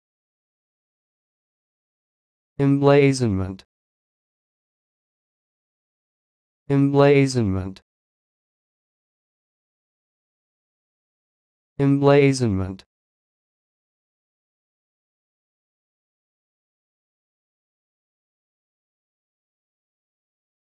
Learn how to say this word: "Emblazonment"
This is the definition of "Emblazonment":
n.  An emblazoning.